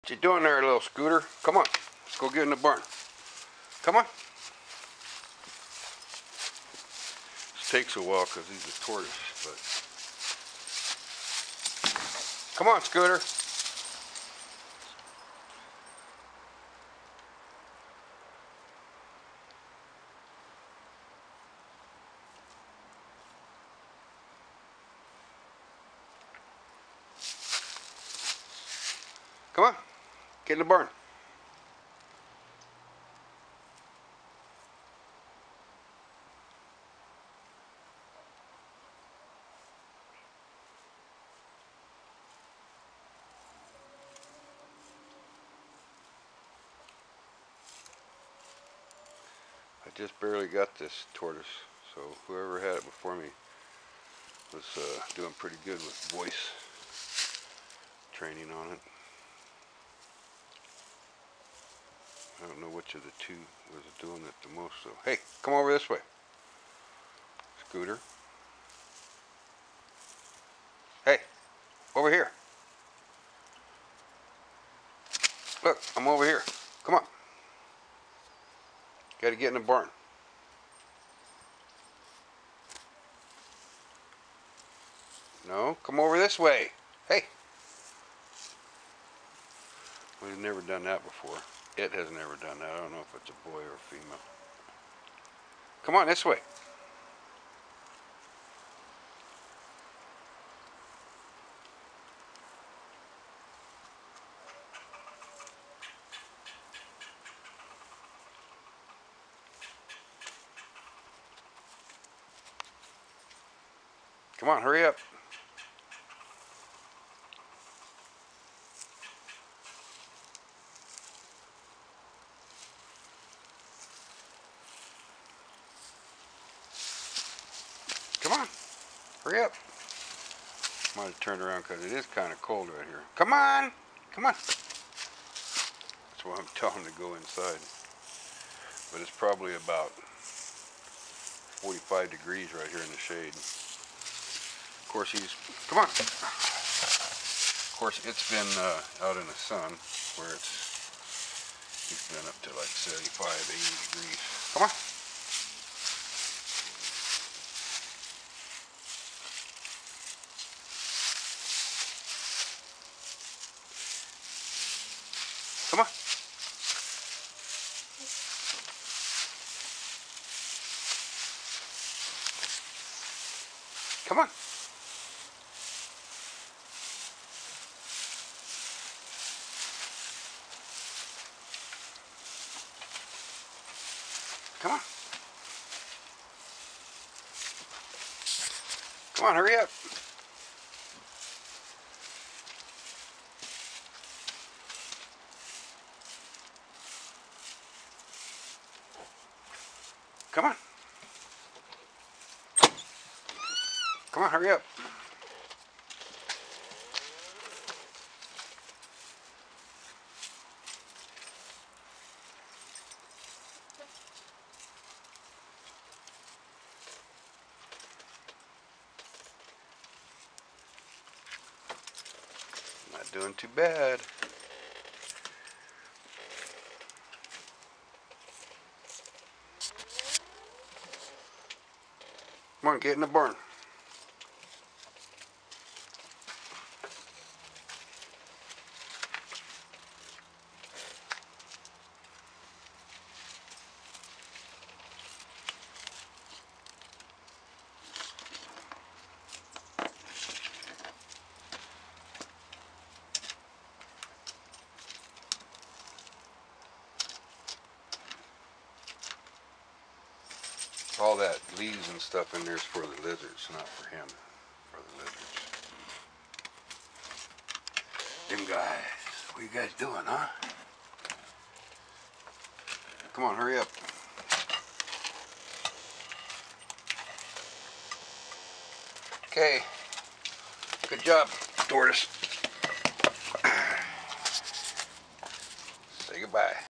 Thats One Smart Turtle, er, Tortoise! 3
I just took in a sulcate about 8yrs old about 3 weeks ago. This tortoise has learned to come back to the lizard barn after eating grass all day. I am uploading 3 videos on this channel of 3 different days, but he does it everyday!